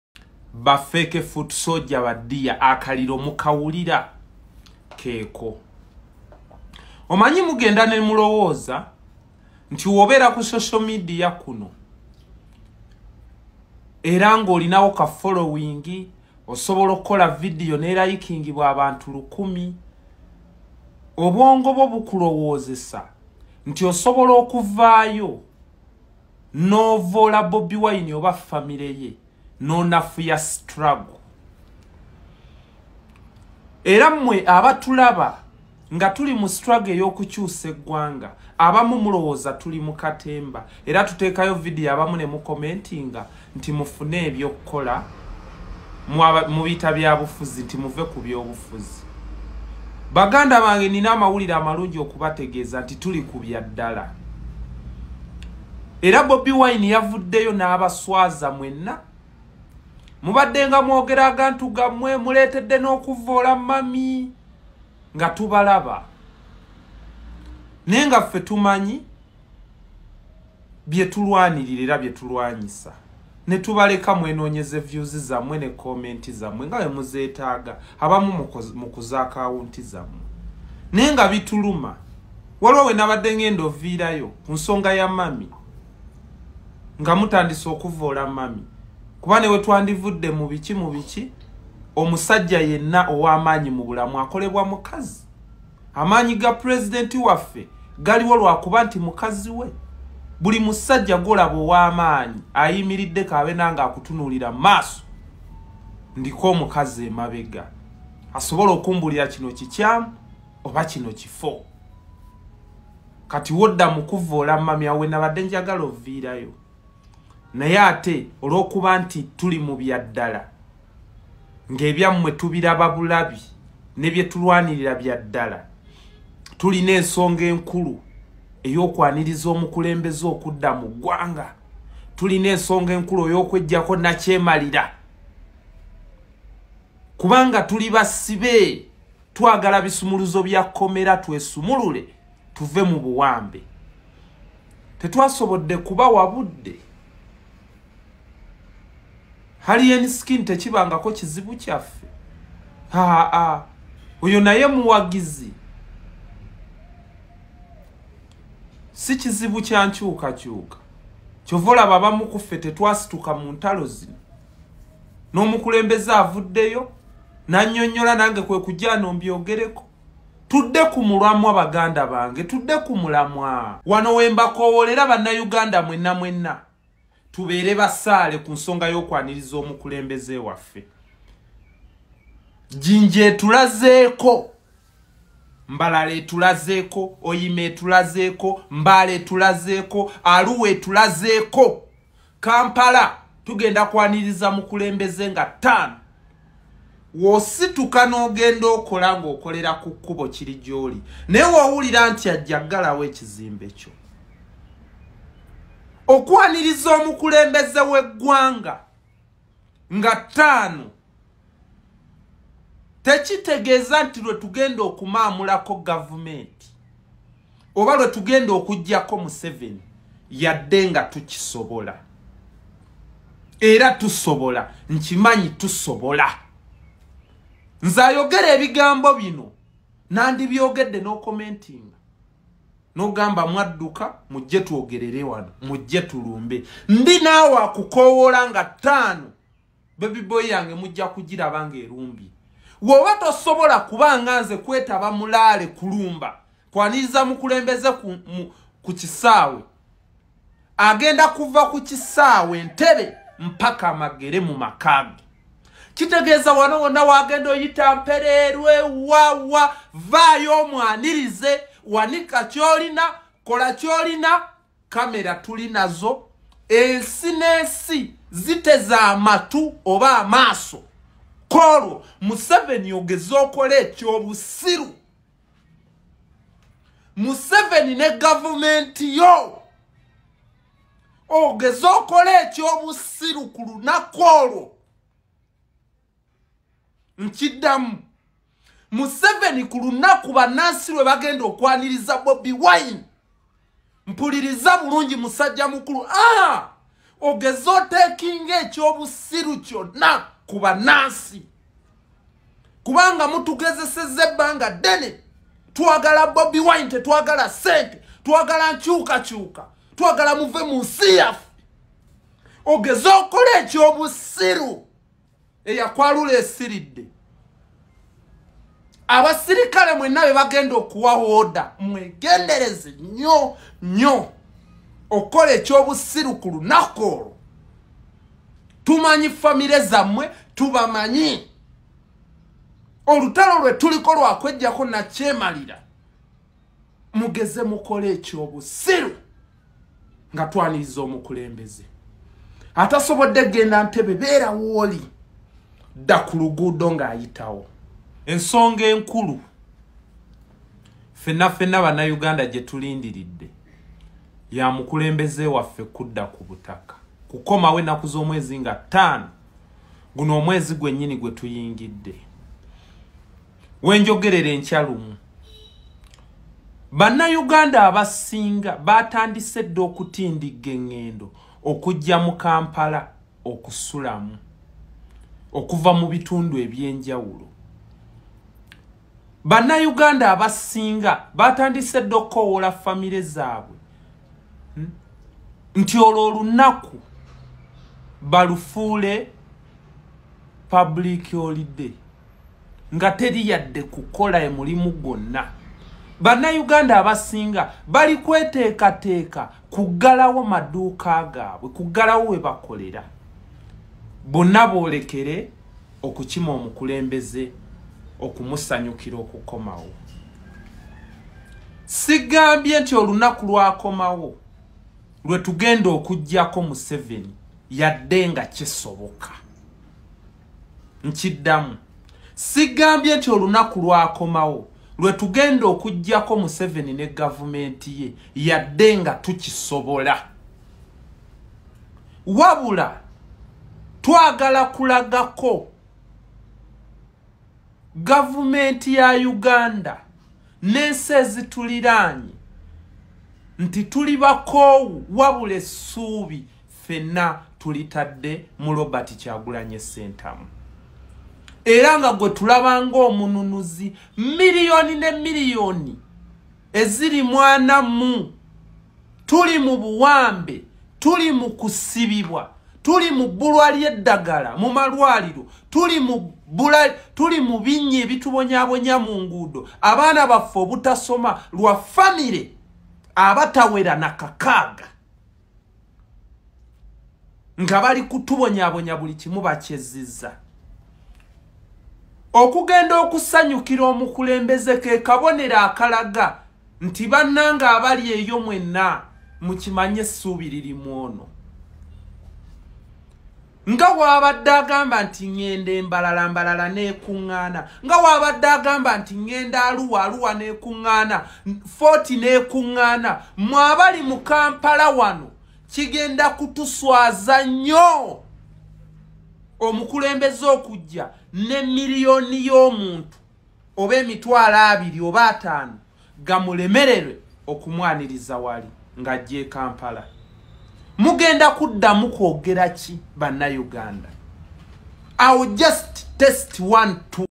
The worst mistake is M7 arresting Bobi Wine at Entebbe Airport, Ugandans are ready for anything...
The worst mistake is M7 arresting Bobi Wine at Entebbe Airport, Ugandans are ready for anything - Sammy